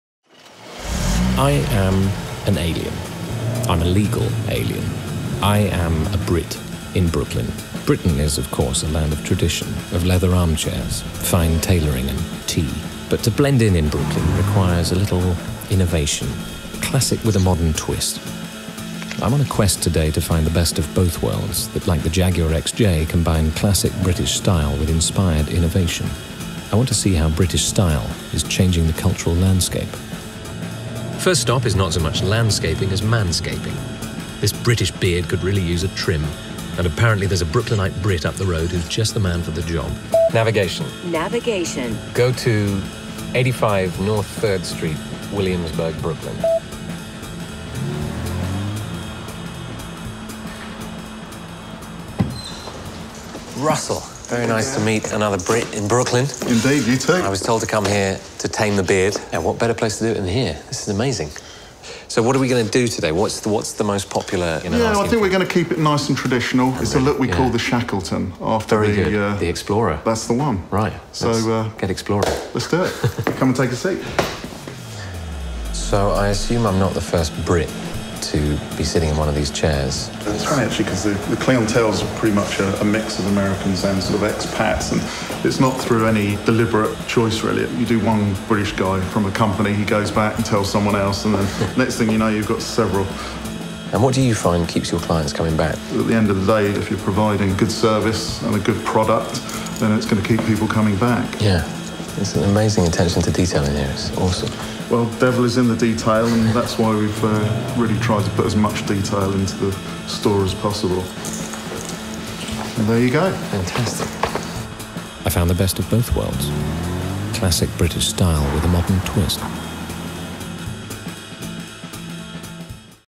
Dan Stevens: Agent of Influence ¦ Best of Grooming ¦ JaguarUSA
Being a Brit in Brooklyn, Dan Stevens is on the search for the best of both worlds – classic British style with inspired innovation - in a Jaguar XJ. In this episode, he visits Ludlow Blunt.